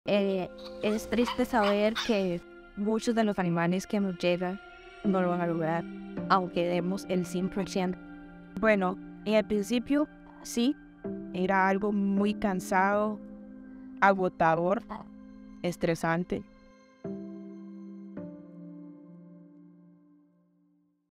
You NEED To Watch This Inspiring Documentary...
FULL VERSION ON YOUTUBE NOW! (LINK IN COMMENTS)

Yesterday, we unveiled a powerful documentary that turns the spotlight onto the inspiring women of IAR Costa Rica Center, who work tirelessly to save the lives of injured and orphaned animals in need. Their untold stories of bravery, compassion, and relentless dedication deserve your attention!